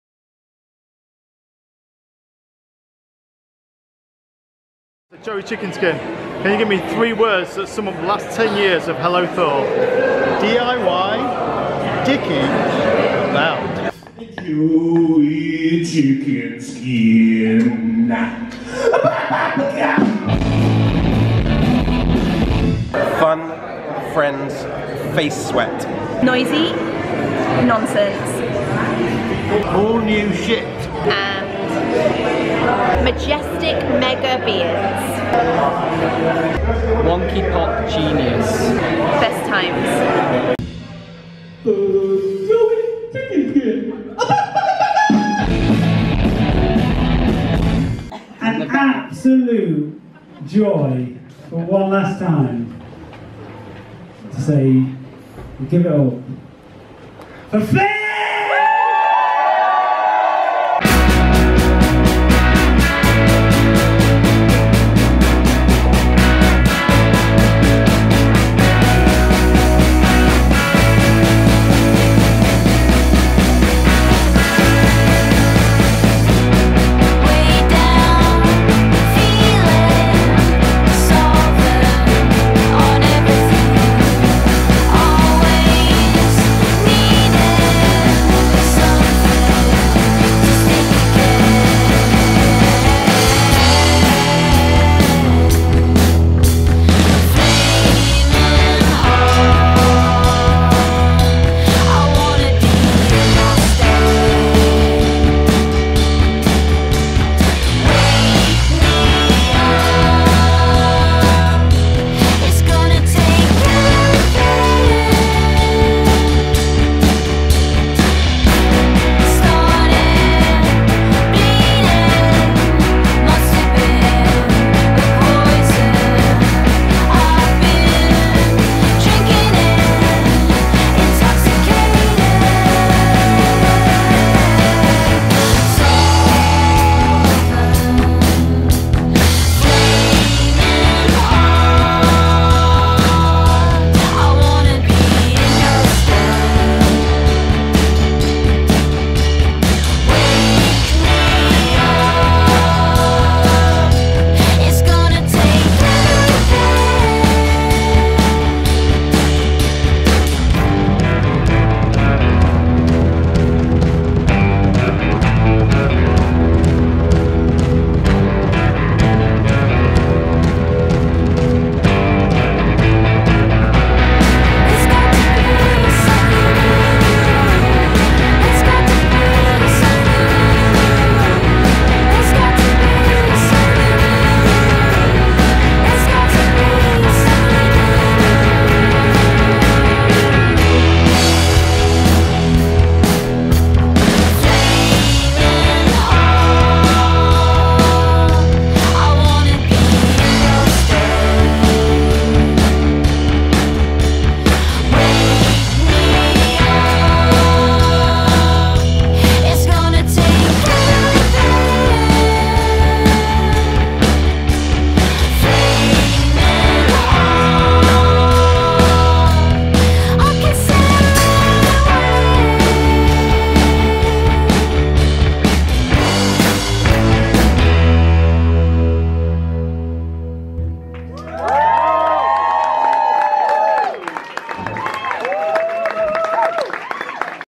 Happy Birthday Thor! HT10 Anniversary weekender - by David Lilley
In mid-May 2019 Hello Thor Records celebrate 10 years of releasing incredible music, throwing awesome parties and goofing around with one last gig. Two last gigs actually.

On Saturday 18 May filmmaker David Lilley popped along and captured Joey Chickenskin & the Sunshine Band, Sad Eyes, Neoangin, We Show Up On Radar, Ultras, Moscow Youth Cult and FISTS.

This sums up the night, the weekend and the last 10 years of Hello Thor pretty well. If you like it, check out the music, form a band, start your own DIY night/label/collective etc - it's a right laugh! x